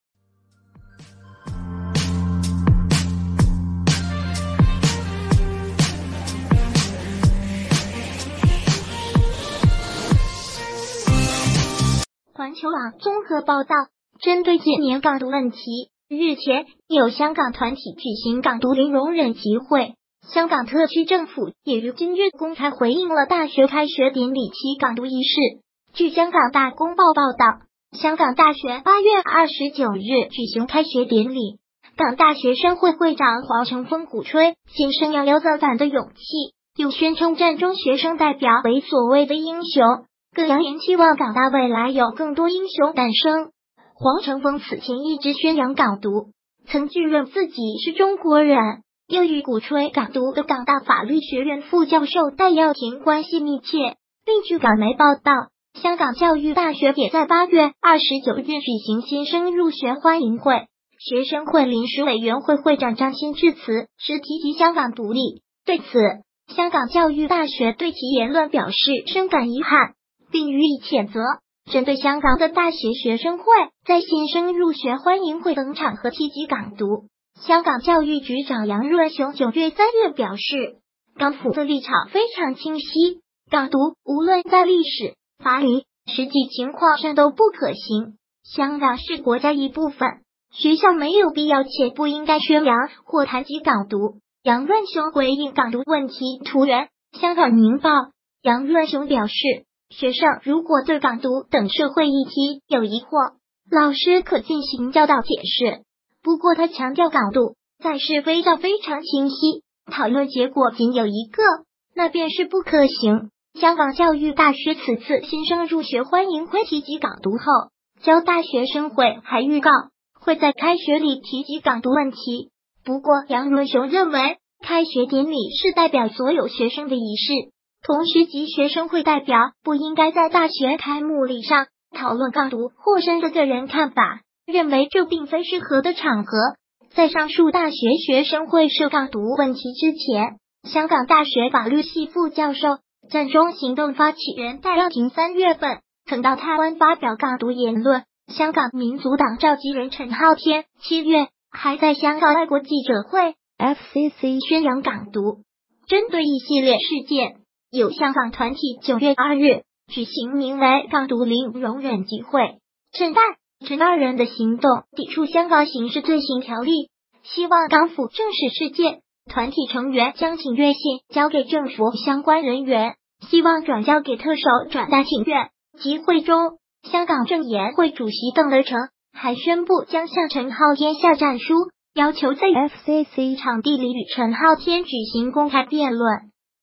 香港大学开学典礼公然提“港独” 香港教育局长严厉回应
【环球网综合报道】针对近年“港独”问题，日前有香港团体举行“港独零容忍”集会。香港特区政府也于今日公开回应了大学开学典礼提“港独”一事。
            据香港《大公报》报道，香港大学8月29日举行开学典礼，港大学生会会长黄程锋鼓吹新生要有“造反的勇气”，又宣称“占中”学生代表为所谓的“英雄”，更扬言期望港大未来有更多“英雄”诞生。
            黄程锋此前一直宣扬“港独”，曾拒认自己是中国人，又与鼓吹“港独”的港大法律学院副教授戴耀廷关系密切。
            另据港媒报道，香港教育大学也在8月29日举行“新生入学欢迎会”，学生会临时委员会会长张鑫致辞时提及“香港独立”。对此，香港教育大学对其言论表示“深感遗憾，并予以谴责”。
            针对香港的大学学生会在“新生入学欢迎会”等场合提及“港独”，香港教育局长杨润雄9月3日表示，港府的立场非常清晰，“港独”无论在历史、法理、实际情况上都不可行，香港是国家一部分，学校没有必要且不应该宣扬或谈及“港独”。
            杨润雄回应“港独”问题（图源：香港《明报》）